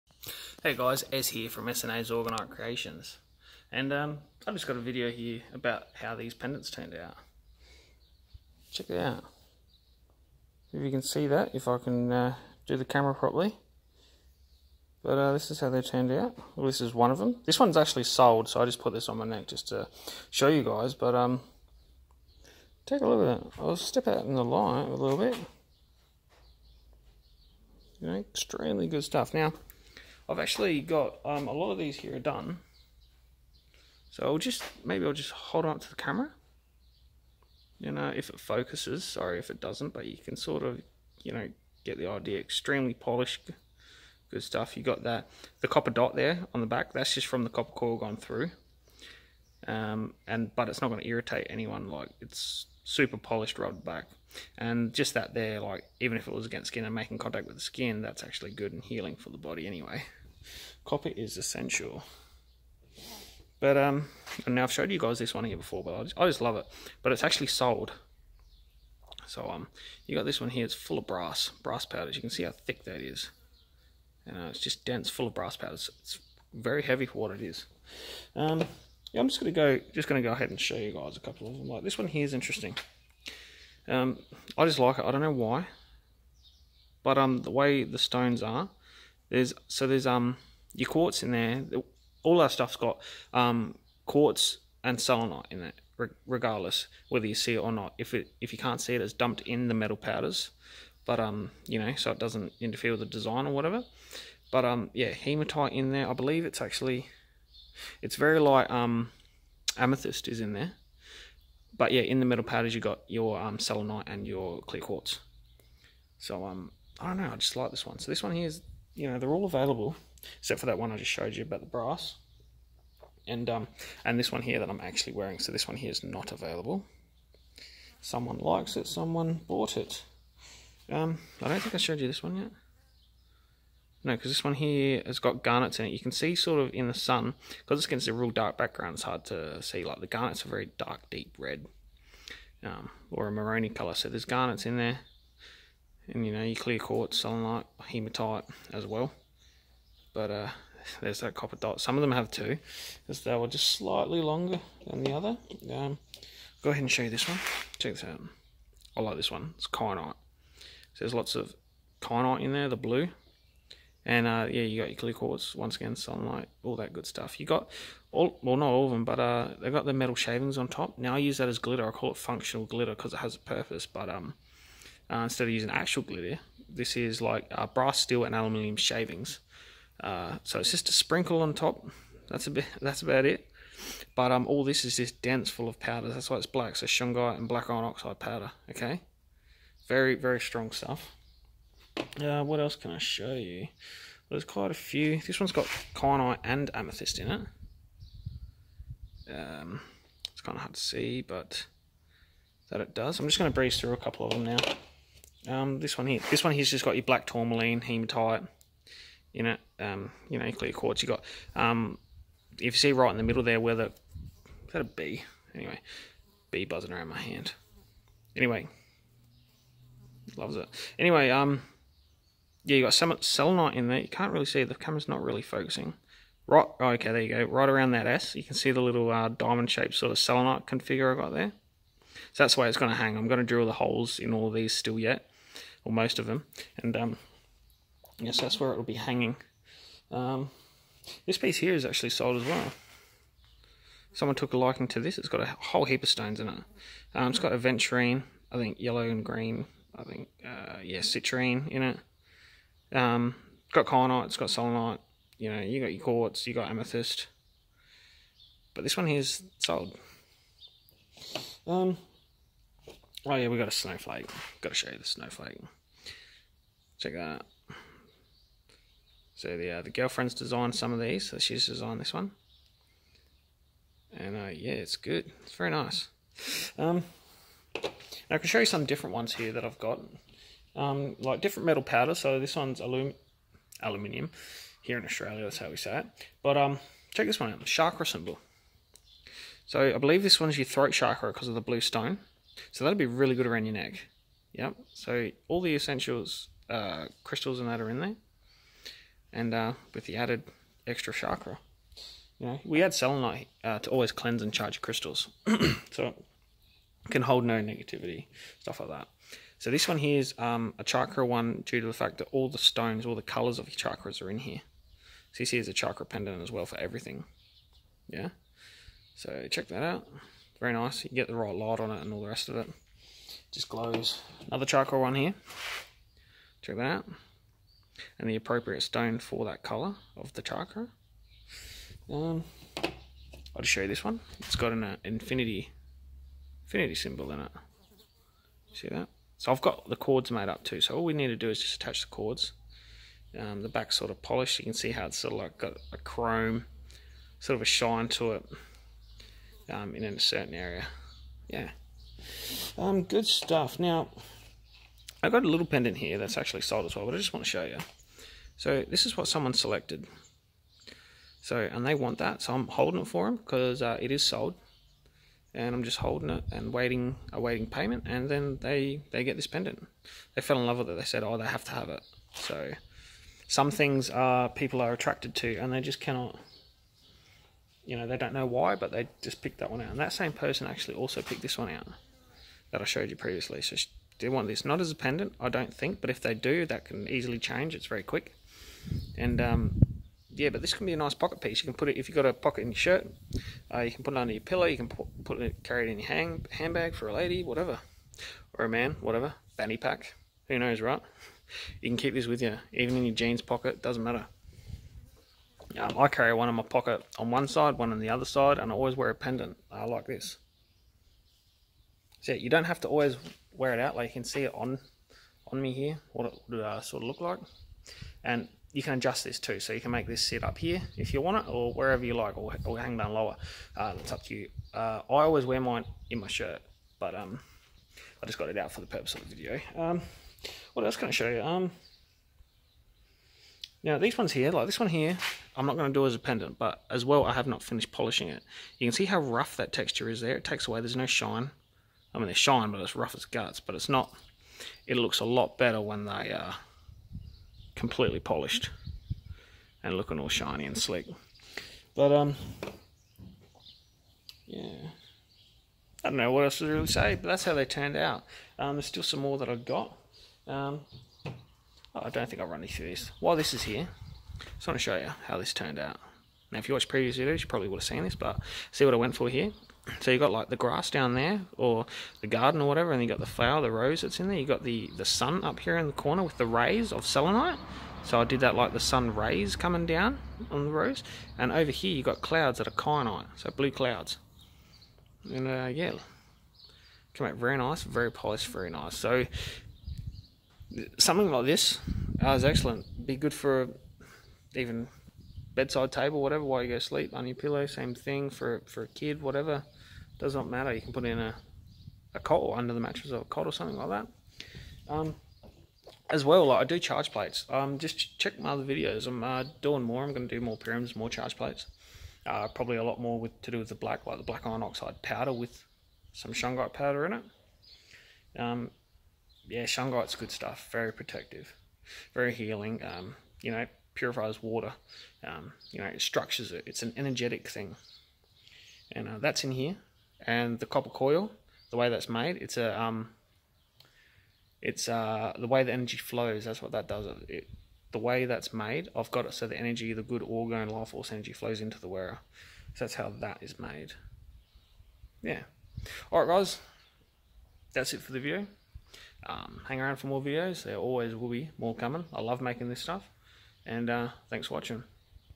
Shungite & Iron Oxide ORGONITE Pendants!!!
New powerful orgonite pendants, all polished and ready for their new owners